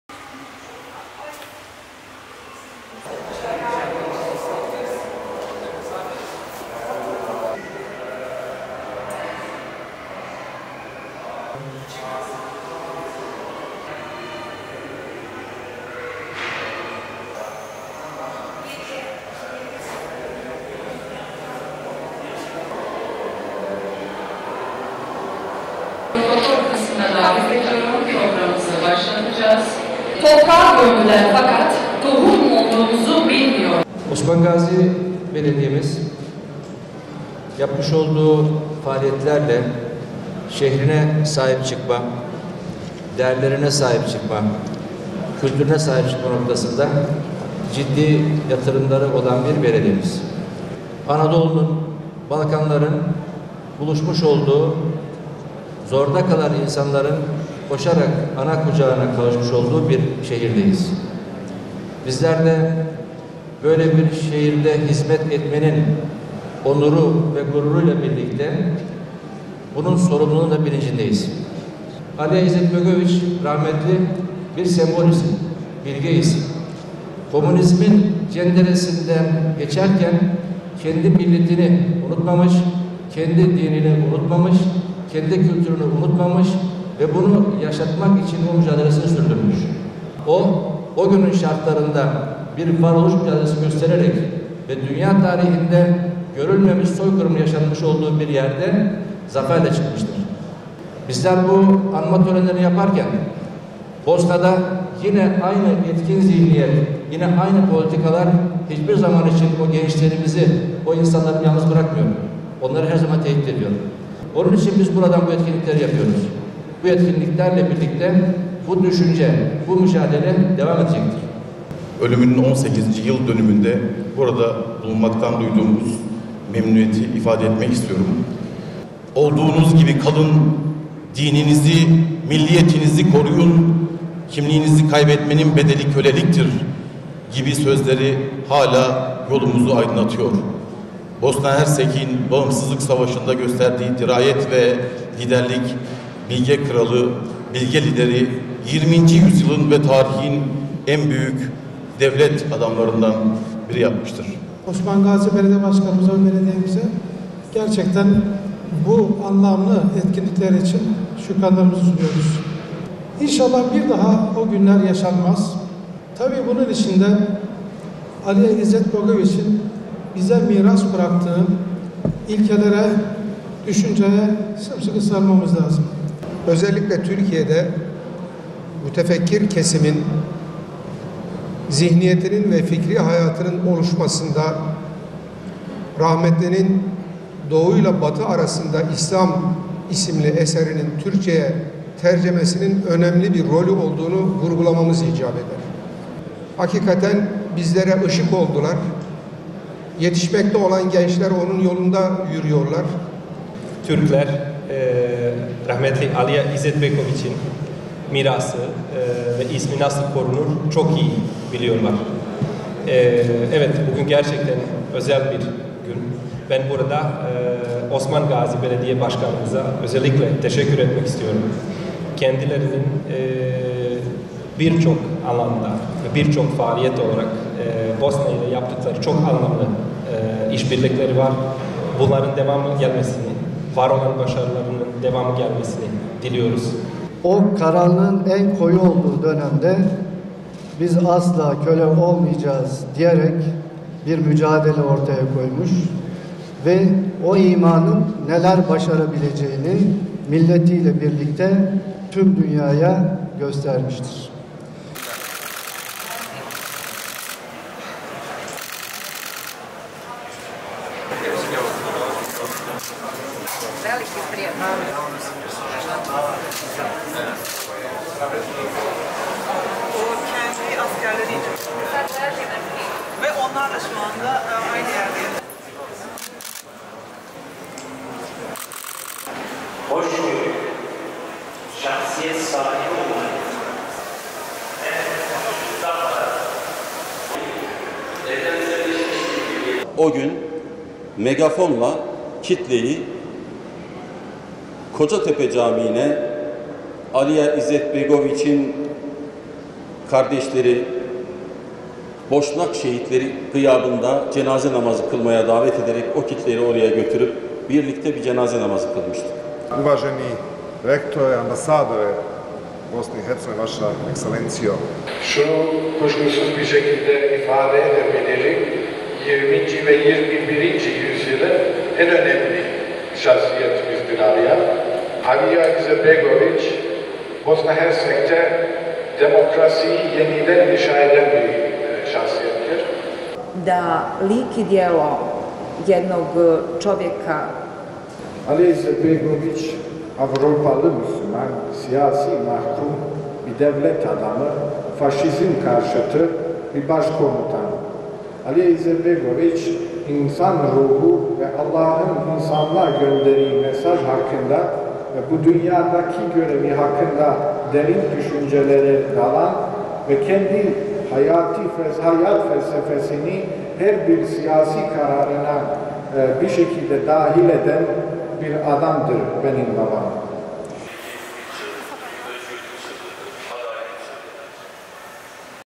Bilge Kral’a Osmangazi Vefası
Osmangazi Belediye Başkanı Mustafa Dündar’ın ev sahipliğinde gerçekleşen serginin açılış törenine; Aliya’nın kızı Lejla Akšamija’nın yanı sıra AK Parti Bursa Milletvekili Refik Özen, Bosna Hersek Ankara Büyükelçisi Adis Alagiç, Uludağ Üniversitesi Rektörü Prof. Dr. Ahmet Saim Kılavuz, Bursa Teknik Üniversitesi Rektörü Prof. Dr. Arif Karademir, Bursa Vali Yardımcısı Mustafa Gündoğan, Bursa Büyükşehir Belediye Başkan Vekili Murat Demir ile akademisyenler katıldı. Katılımcılara, Bosna savaşı ve Bosna halkının acılarının simgesi olan ‘mavi kelebek’ rozeti hediye edildi.